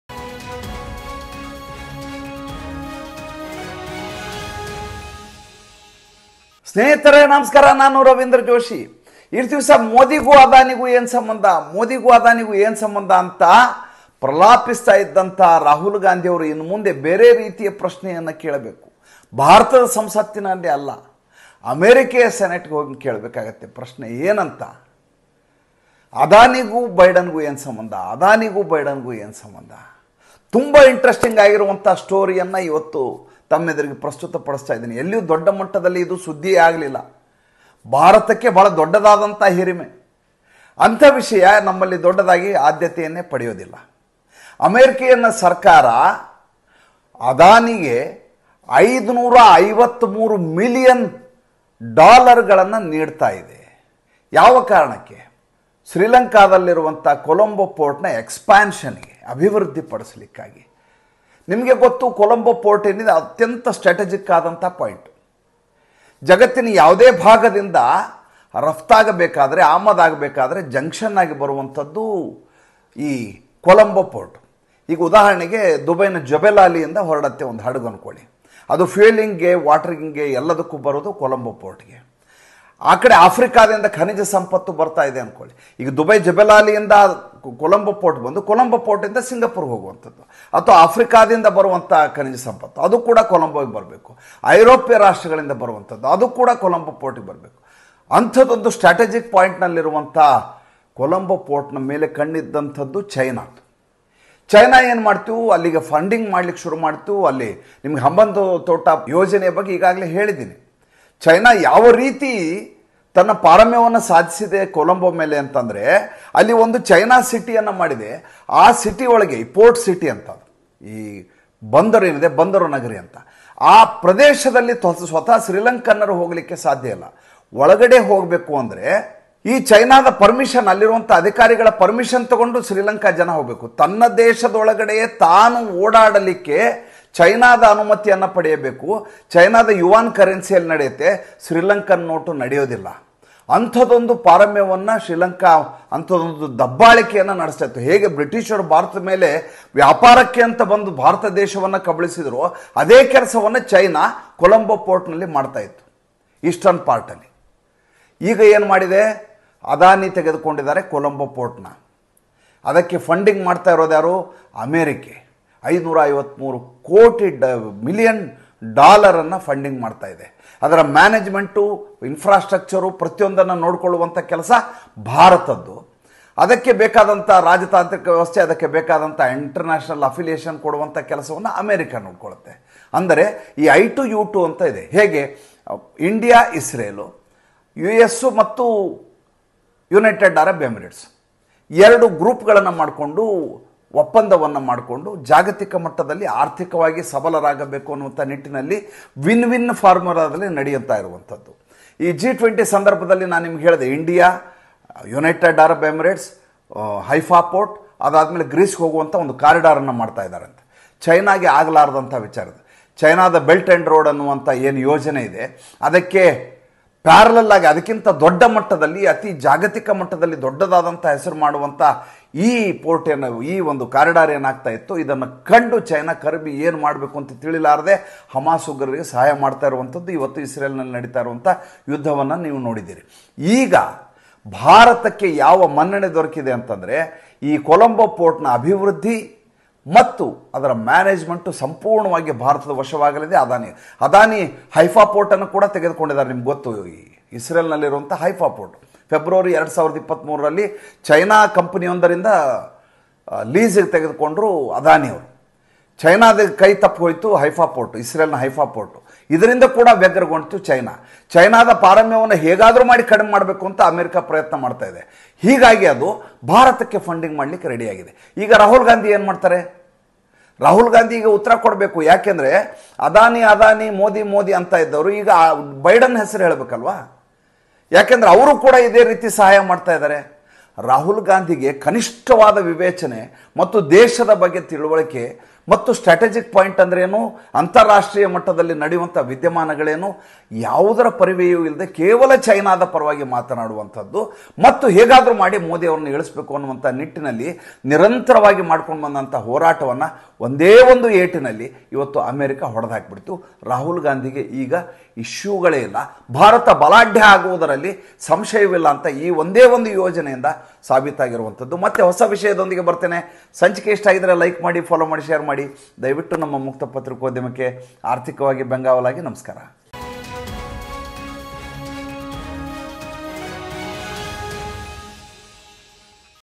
ಬೈಡನ್ ಕೊಟ್ಟ ಏಟಿಗೆ ರಾಹುಲ್ ಮತ್ತು ಚೈನಾ ಮುಟ್ಟಿ ನೋಡಿಕೊಳ್ಳುವ ಹಾಗಾಯಿತು U.S FUNDS ADANI FOR COLOMBO PORT
ಬೈಡನ್ ಕೊಟ್ಟ ಏಟಿಗೆ ರಾಹುಲ್ ಮತ್ತು ಚೈನಾ ಮುಟ್ಟಿ ನೋಡಿಕೊಳ್ಳುವ ಹಾಗಾಯಿತು
 U.S FUNDS ADANI FOR COLOMBO PORT
RavindraJoshiCreationsJoin this channel to get access to perks